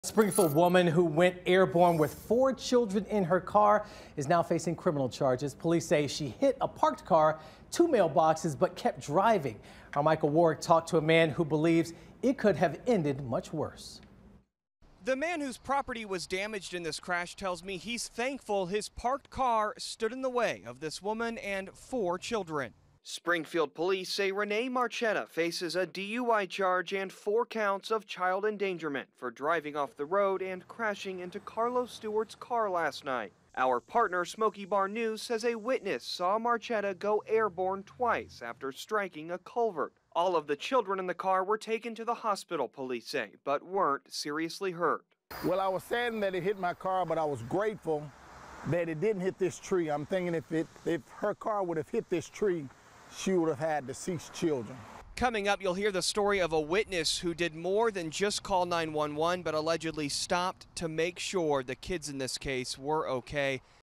Woman's car goes airborne with her kids inside
The mother and the young children were not injured in the crash.